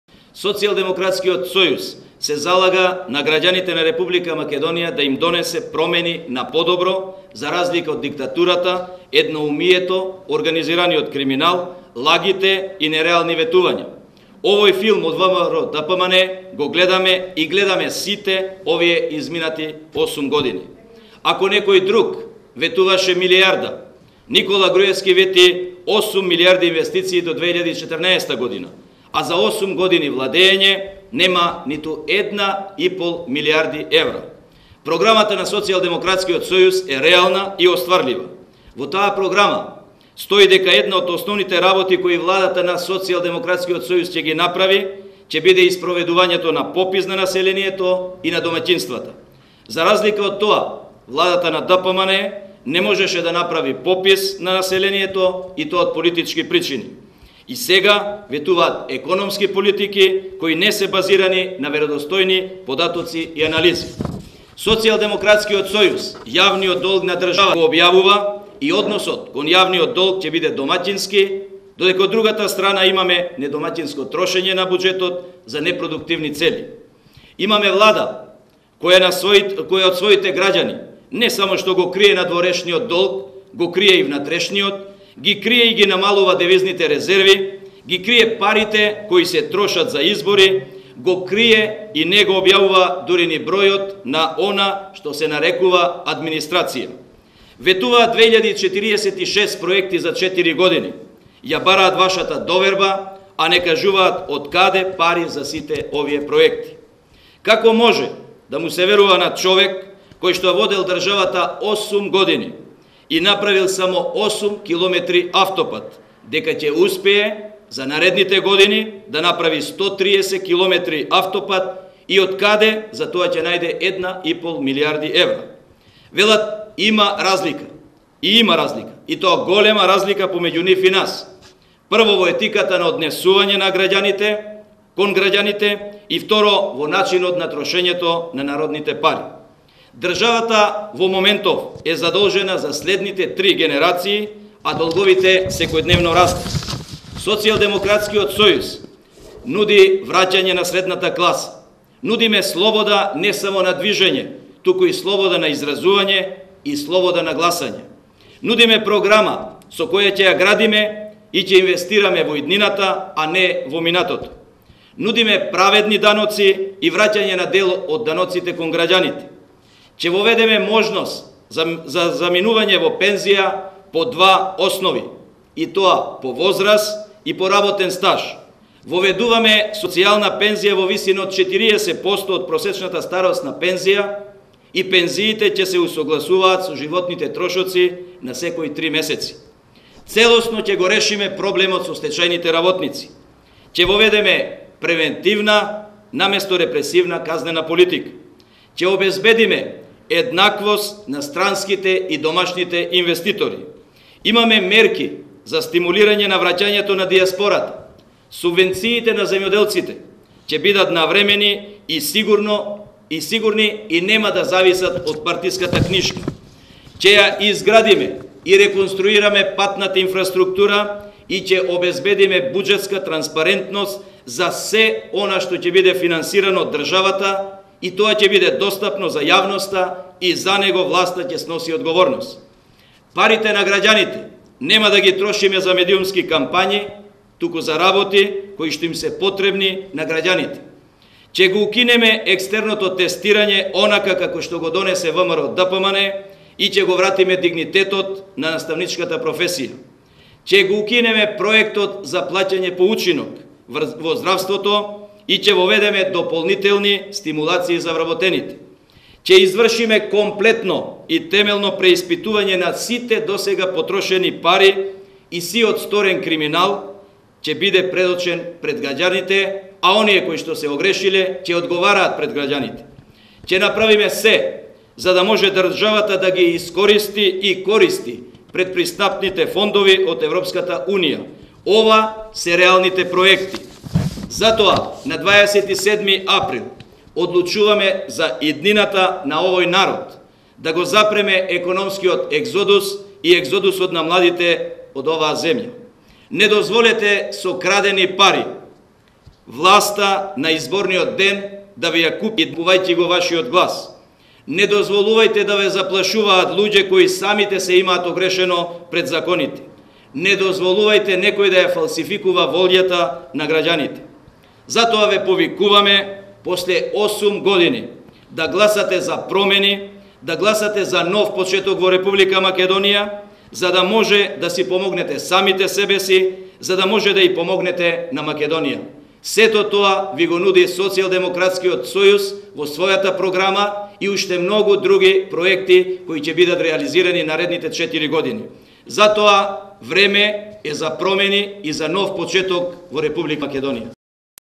Пауновски: Програмата на СДСМ е реална и остварлива
Носителот на листата во ИЕ 6 од СДСМ Стојко Пауновски на прес конференција ги изложи заложбите на партијата и темелите врз кои се заснива нивната изборна програма